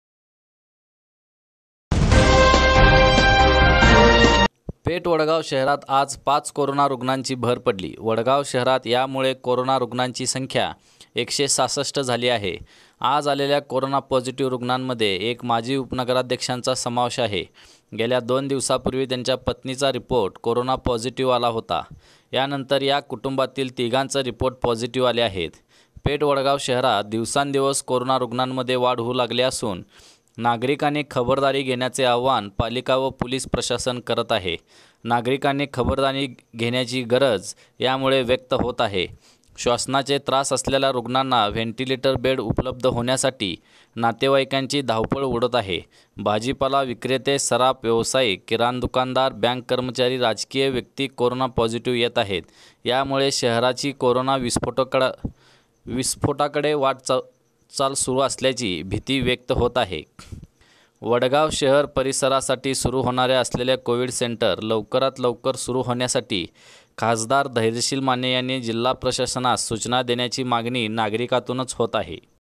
पेठवडगाव : पेठवडगावात पाच कोरोना रुग्णांची भर, शहराची रुग्णसंख्या झाली १६६
पेठवडगाव  : पेठवडगावात पाच कोरोना रुग्णांची भर, शहराची रुग्णसंख्या झाली १६६ :नवा महाराष्ट्र न्यूज